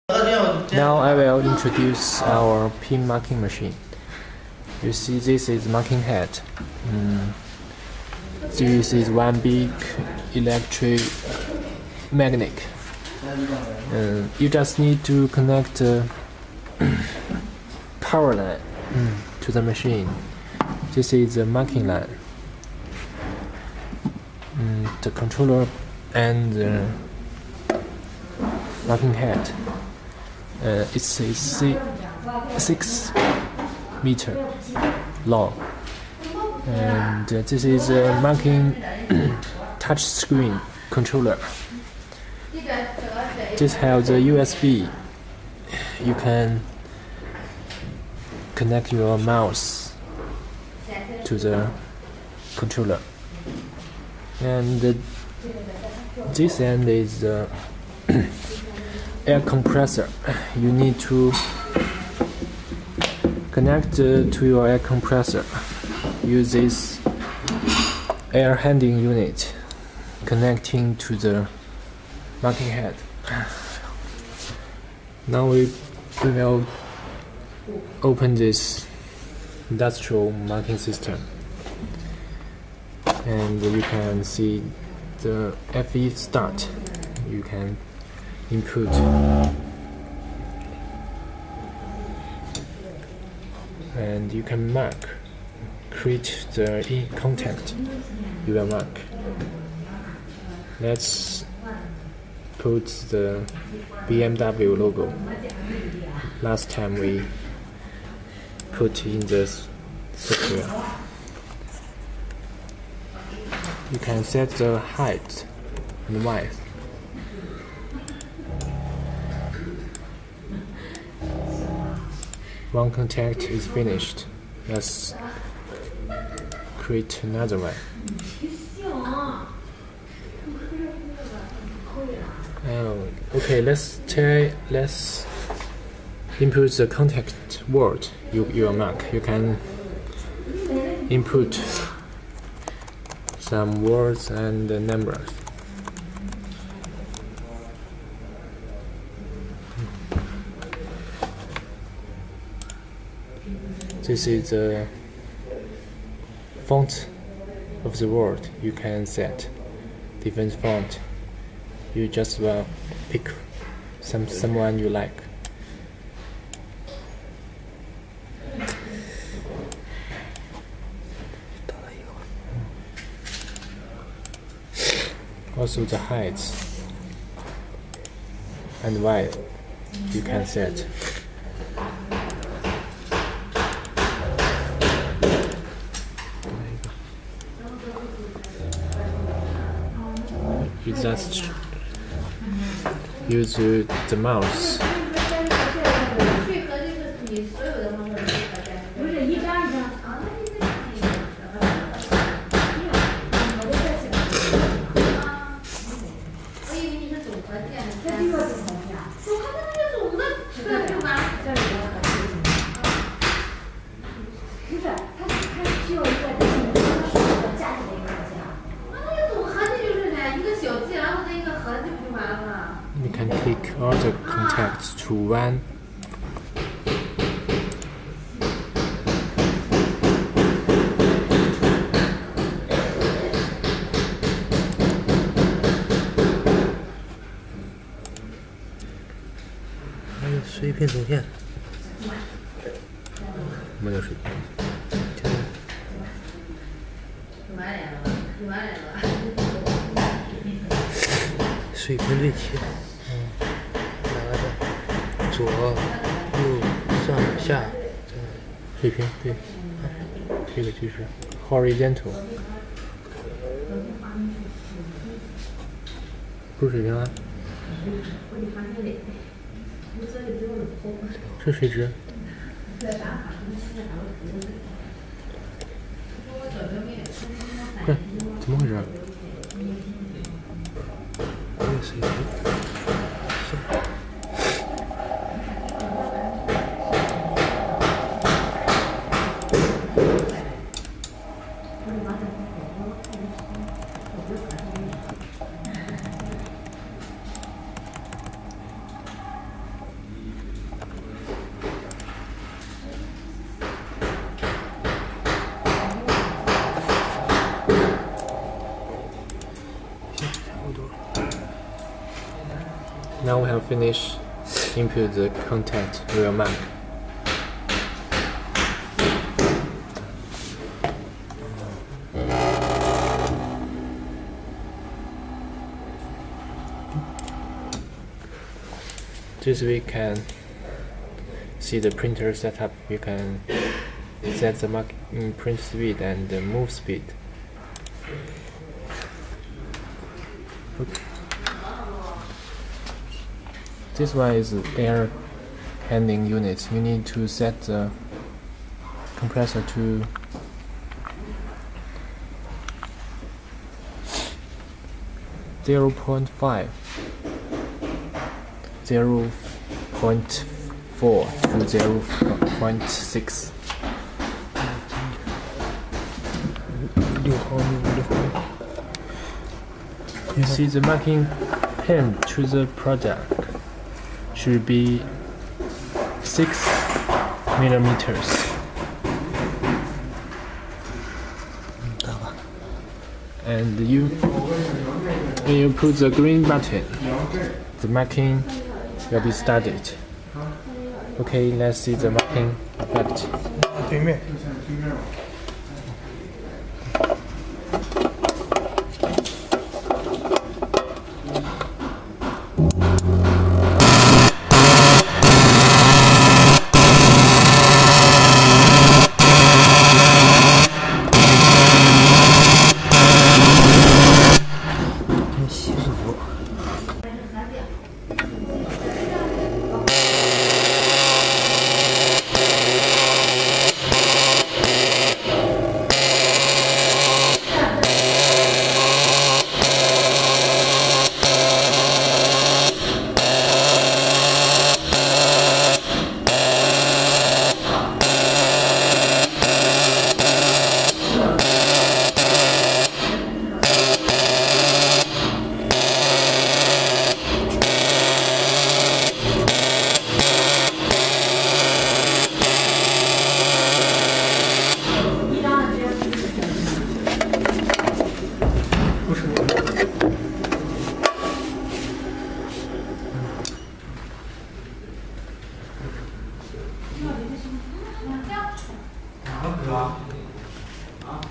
portable Pneumatic dot peen pin marking machine for vin
This portable pneumatic dot peen marking machine with touch screen control box made by SHUNER can mark VIN code,Chassis number,nameplate etc.It will be packaged in one wooden case,gross weight 25kgs,size of package:50*50*50cm,most of our clients like to transport this machine by DHL or TNT express company,Pls send us your detailed destination address,DDU price will be sent to you soonest.